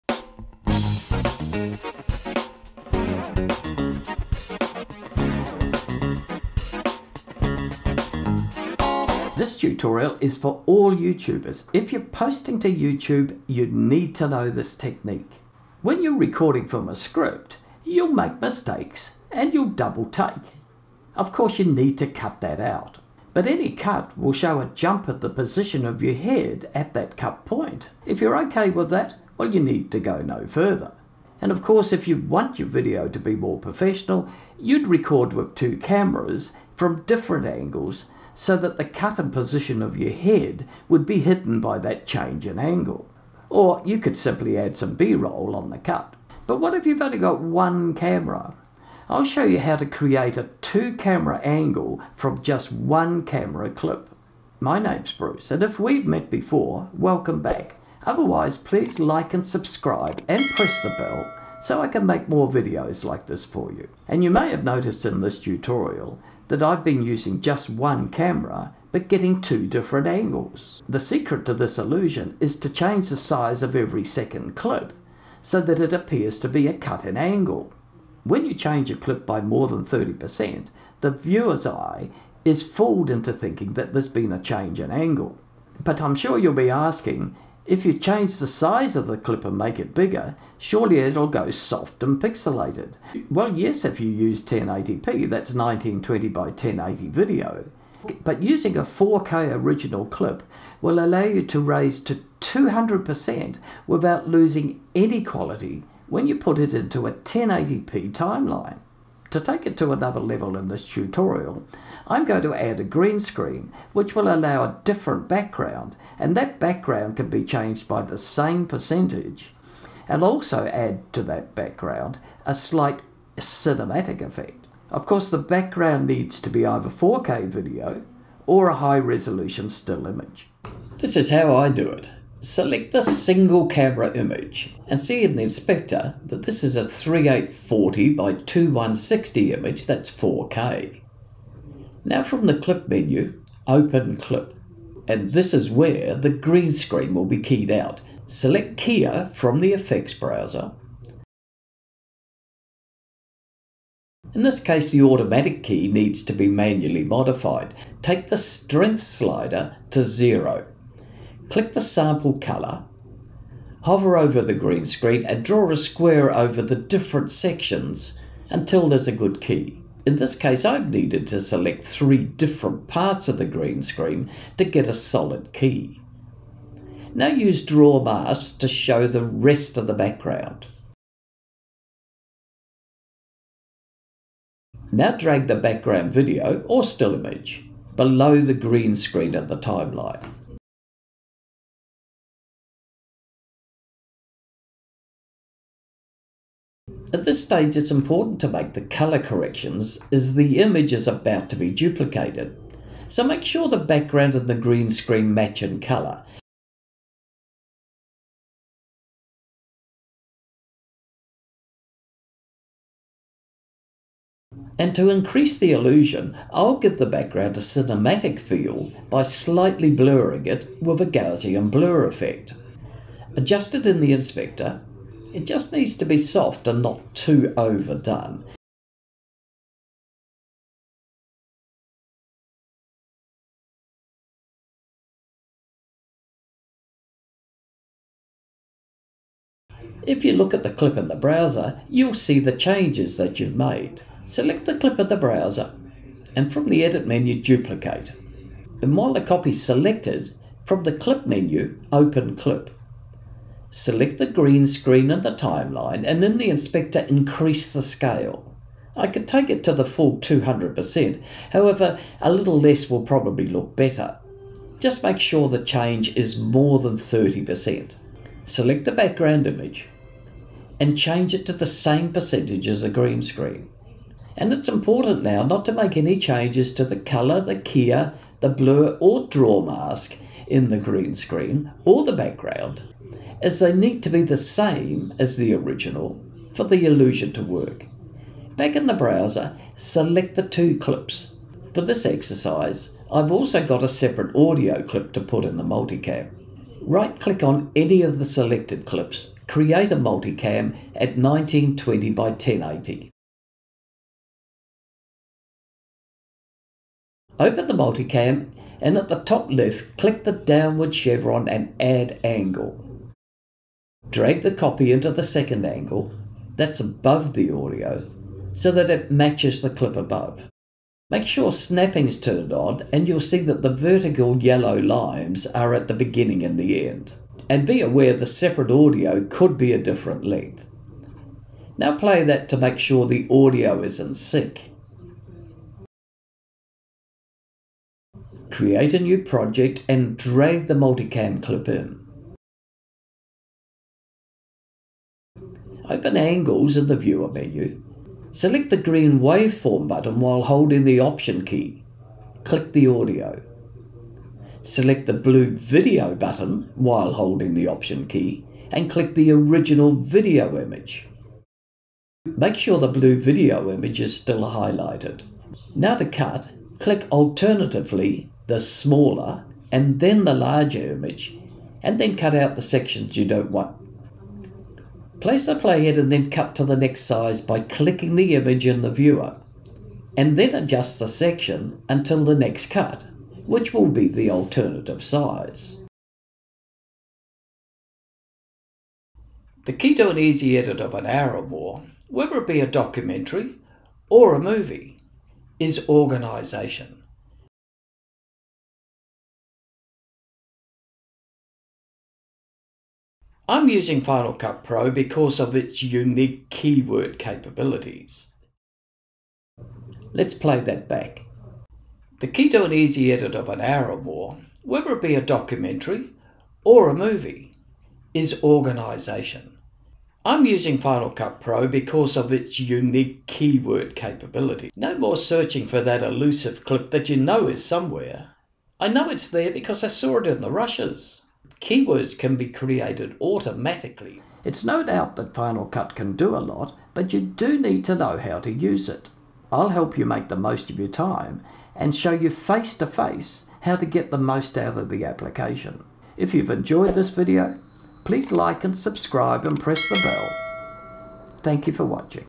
One camera - two camera angles - Multicam Final Cut Pro
Posting to Youtube? You need to know this. tutorial is for all YouTubers.
When you’re recording from a script you’ll make mistakes and double-takes, of course you’ll need to cut them out.
But any cut will show a jump in the position of your head - If you’re OK with that then go no further.
Of course, if you want you video to look more professional you would record with two cameras from different angles so that the cut and position of the head will be hidden by change in angle.
Or you could put some B roll over the cuts.
But what if you have only one camera?
I’ll show yo how to create two camera angles from just the one camera clip?

Beginning: 0:00
Select the single camera image 02:11
Open Clip from the Clip Menu 02:21
Key the green Screen 02:31
Mask the rest of the green screen 02:57
Make all color correction and changes in this clip    03:16
Add a cinematic effect 03:34
Duplicate the original clip    04:05
Make a multicam clip 04:49
Cut the multicam clip be selecting alternative angles 05:38
The completed 2 x angle cut 06:51